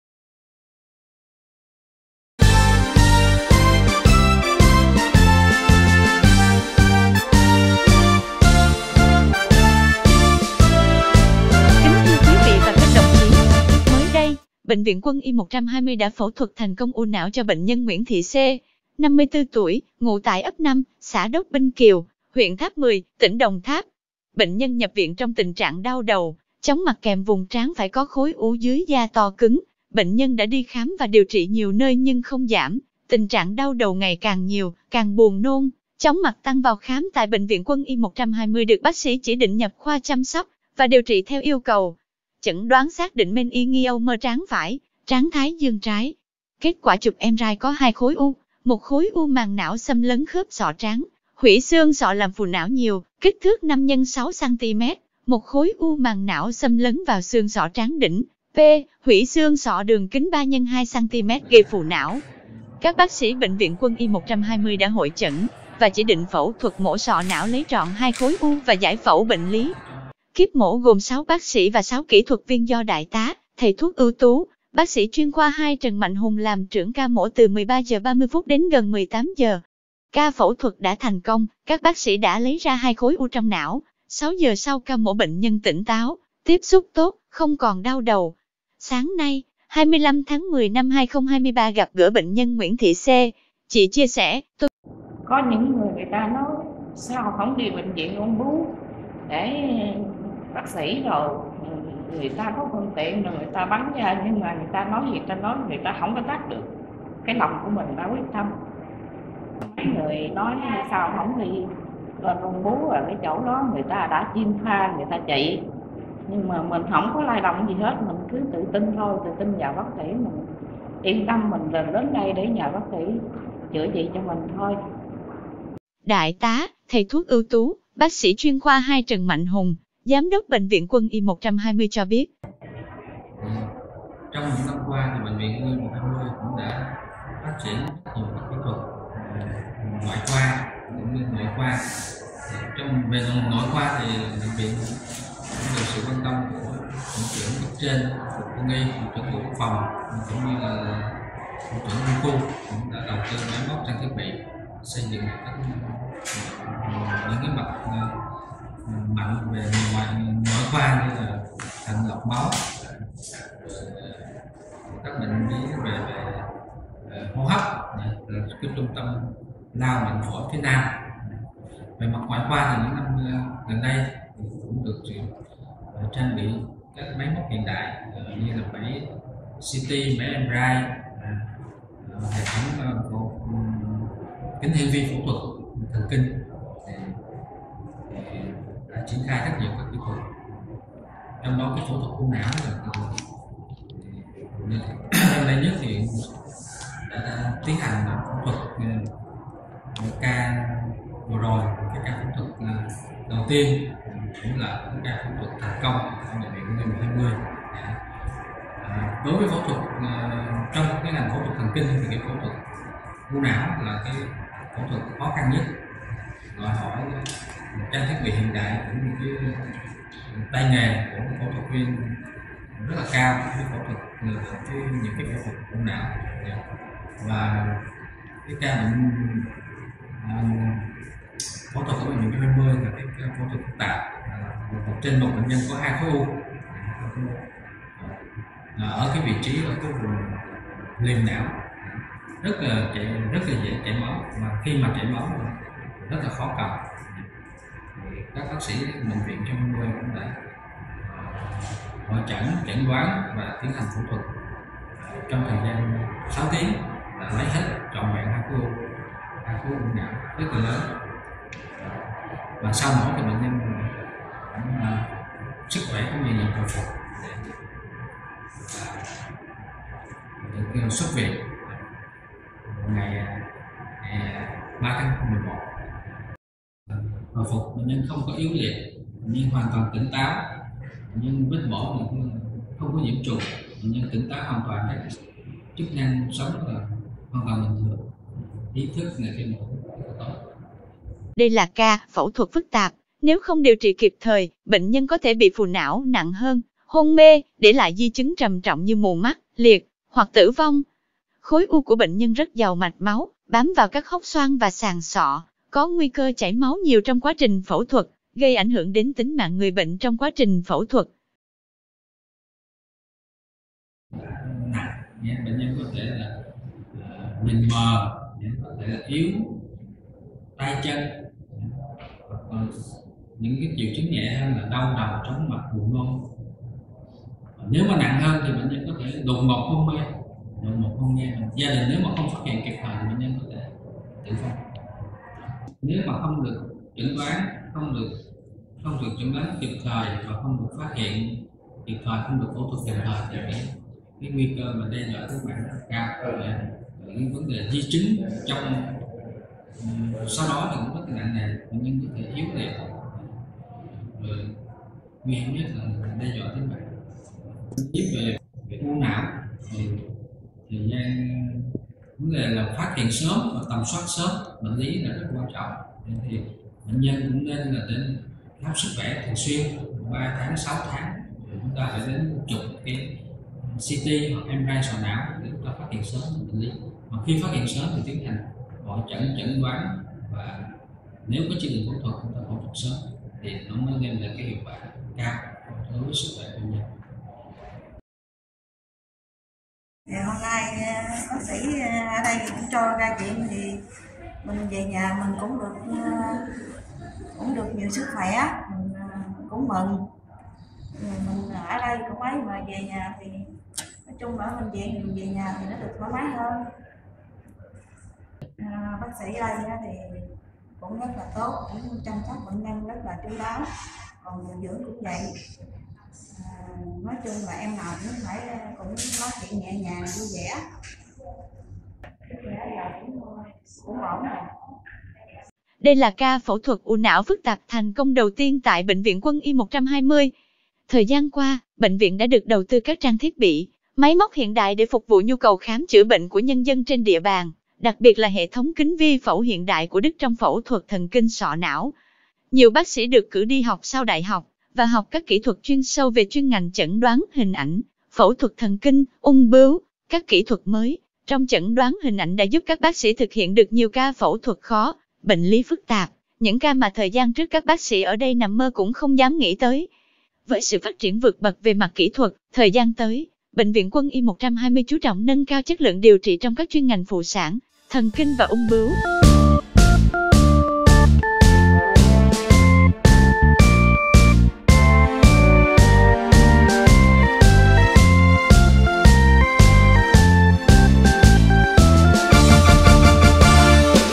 Bệnh viện quân y 120 cứu sống nữ bệnh nhân mang 2 khối u não
Đại tá, Thầy thuốc Ưu tú, BSCK2 Trần Mạnh Hùng – Giám đốc Bệnh viện quân y 120 cho biết: “Đây là ca phẫu thuật phức tạp, Nếu không điều trị kịp thời, bệnh nhân có thể bị phù não nặng hơn, hôn mê, để lại di chứng trầm trọng như mù mắt, liệt… hoặc tử vong. Khối u của bệnh nhân rất giàu mạch máu, bám vào các hốc xoang và sàn sọ, có nguy cơ chảy máu nhiều trong quá trình phẫu thuật, gây ảnh hưởng đến tính mạng người bệnh trong quá trình phẫu thuật”.